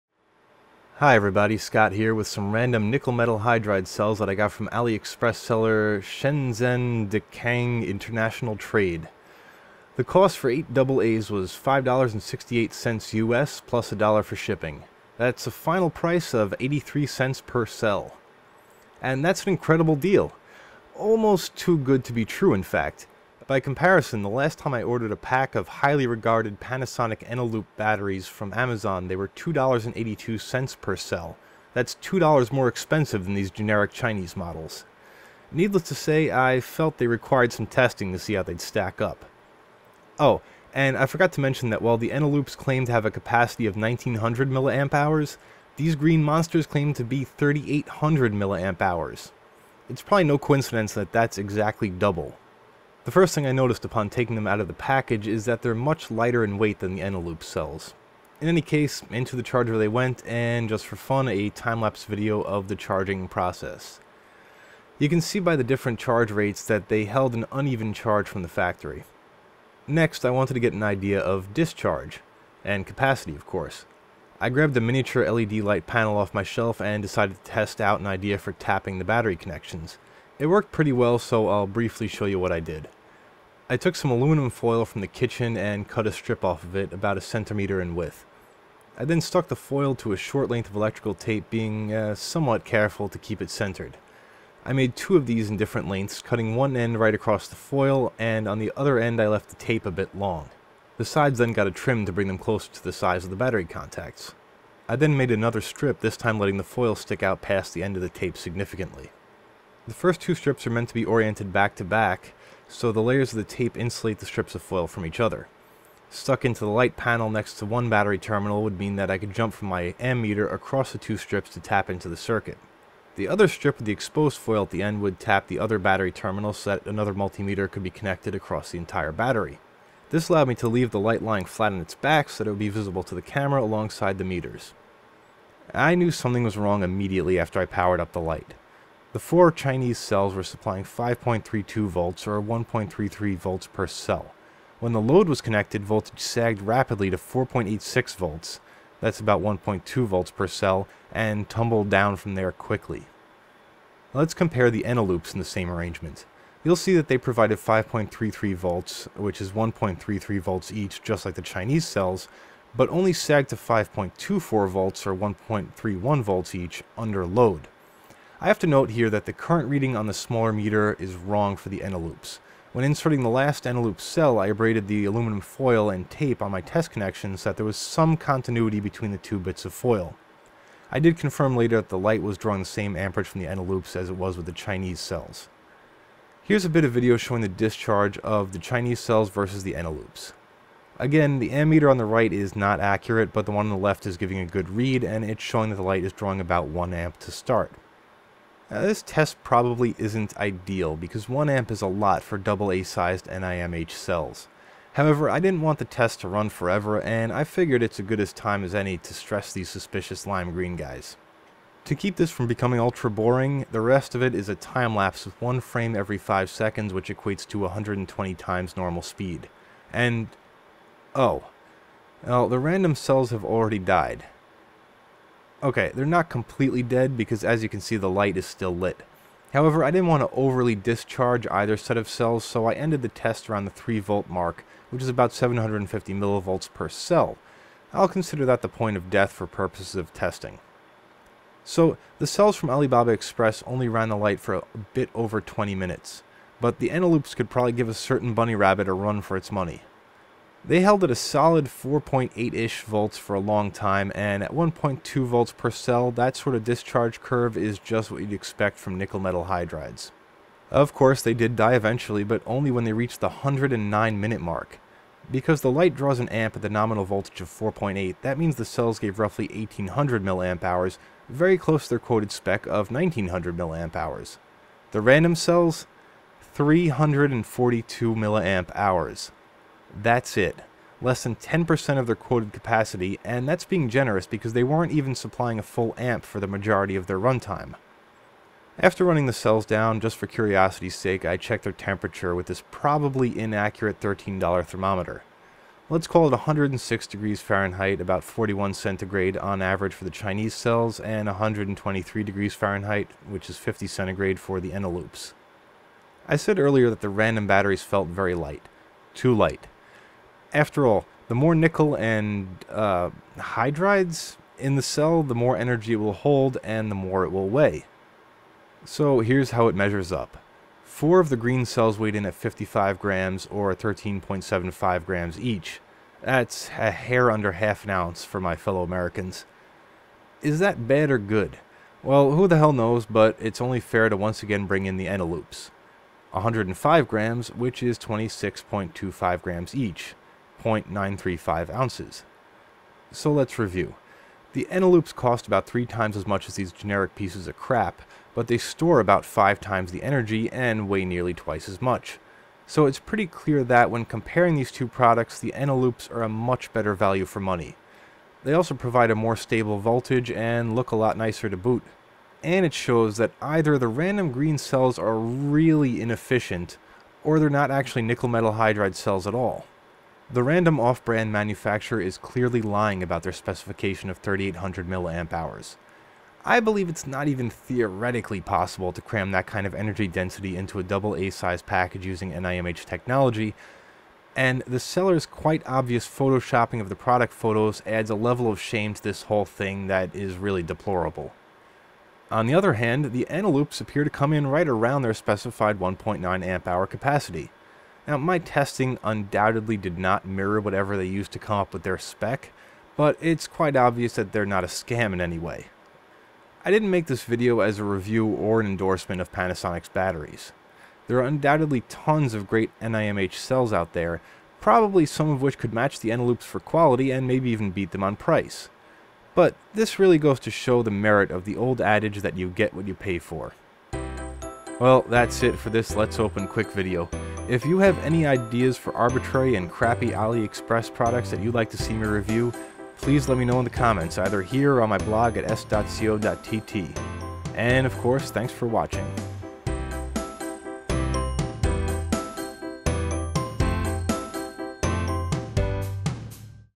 Let's [Quickly] Open: Crappy NiMH Cells from AliExpress vs. Panasonic Eneloop Cells
A look at some possibly-fake random "NiMH" AA cells from AliExpress, comparing them to Panasonic Eneloop cells.

The part about them perhaps not being real NiMH cells?  That's not the weirdest thing.  Unlike most of my AliExpress purchases, these shipped from The Netherlands, despite the seller being called Shenzhen DeKang International Trade.

I absolutely do NOT recommend buying these, but for posterity here is a link to the product page on AliExpress:

I picked them up for US$6.62 including shipping, but as of the time I'm updating this description (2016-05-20) they're going for a ridiculous US$11.44!